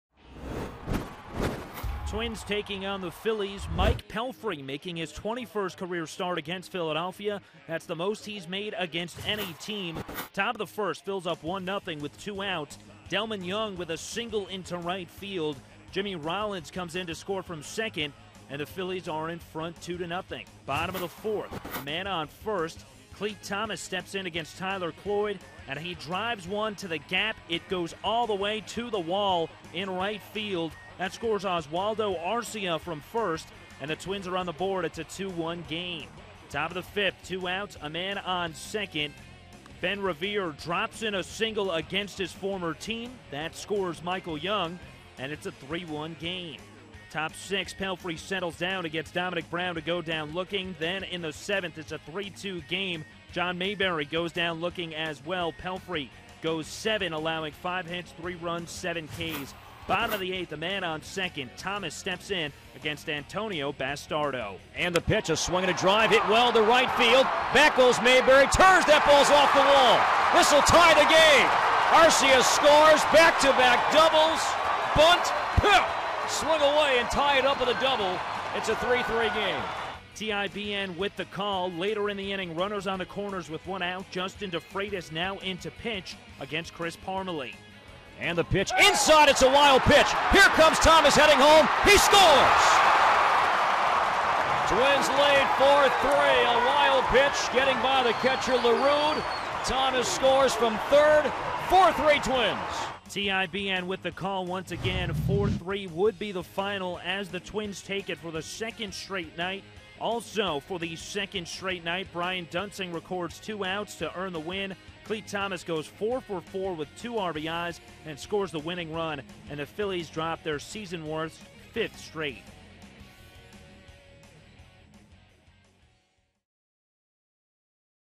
6/12/13: Clete's career night propels Twins to win
Daily Recap: Clete Thomas hit a career-high four hits, including driving in the game-tying RBI and scoring the go-ahead run in the eighth